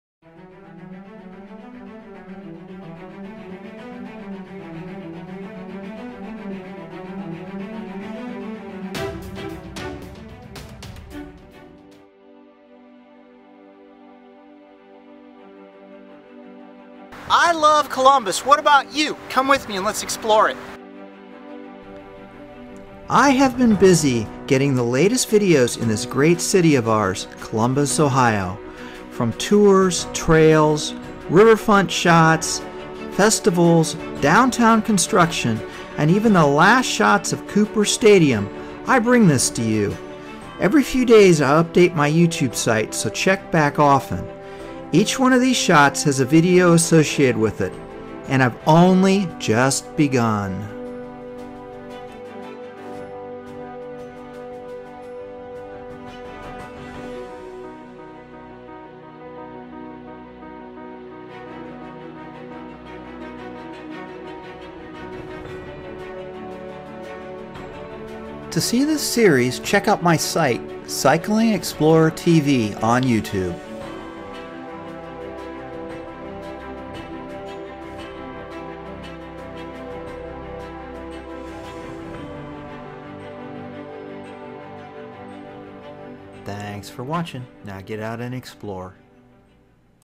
Columbus Ohio Overview
Explorers Are Us - Each shot has a video associated with it about the Columbus area. Whether tours, trails, riverfront, festivals, downtown construction, and even the last shots of Cooper Stadium, I bring this to you.